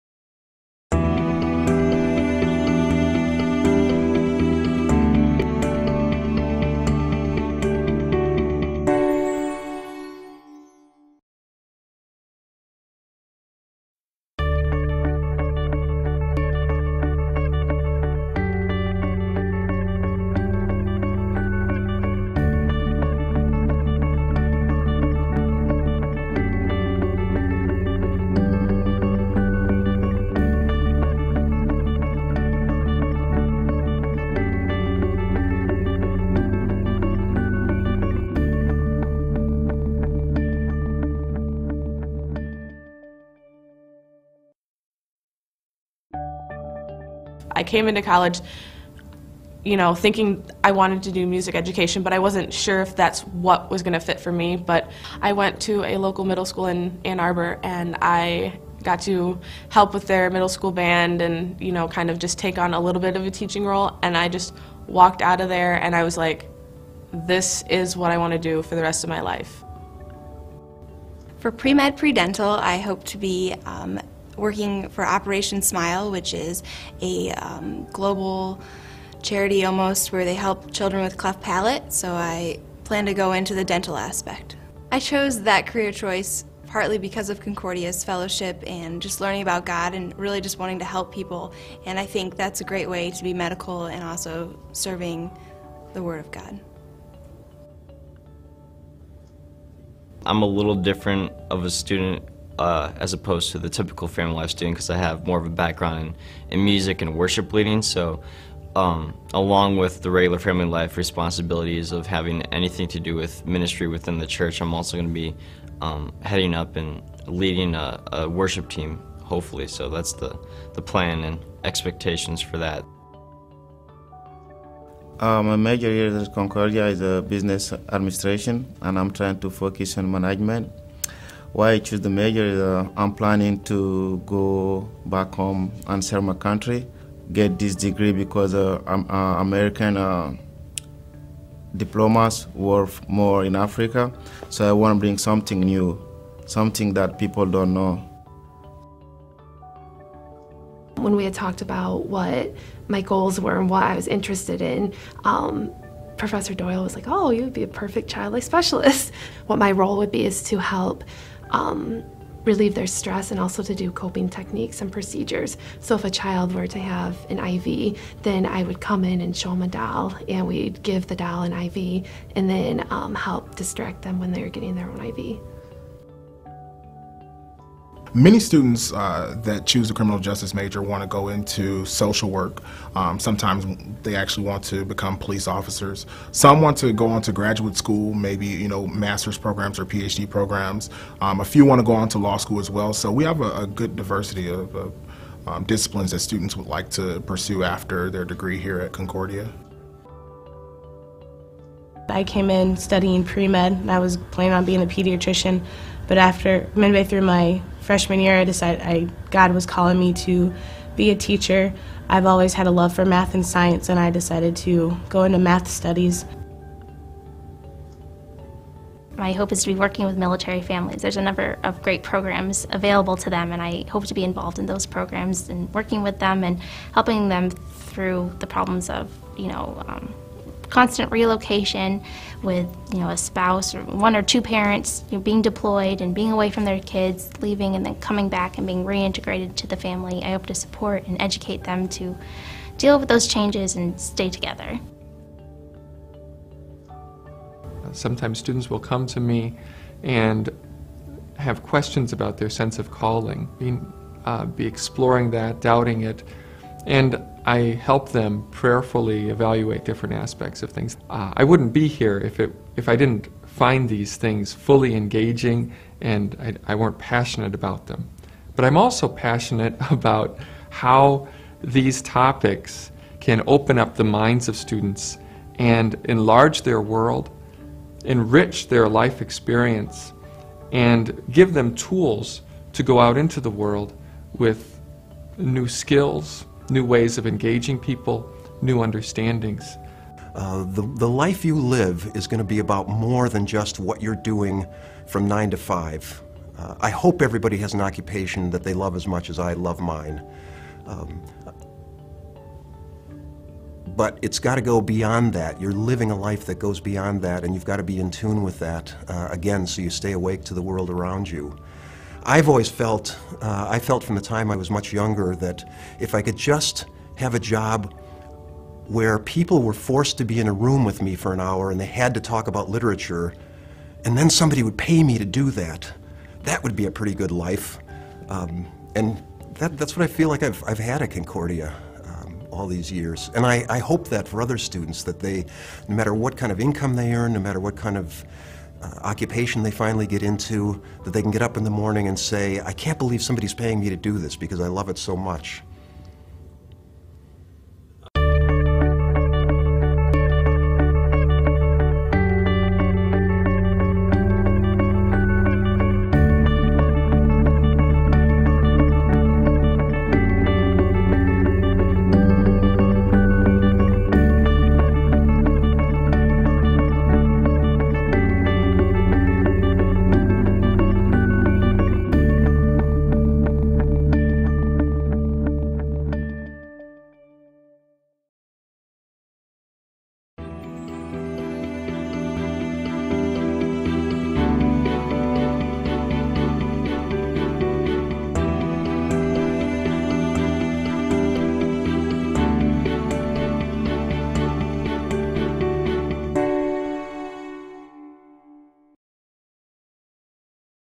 "How Will God Use You?" - Concordia University Ann Arbor
With One Heart is a four-part video series detailing stories of triumph, overcoming adversity, and following God's will for our lives.

"How Will God Use You?" highlights the many ways in which God calls each of us individually for work in His kingdom.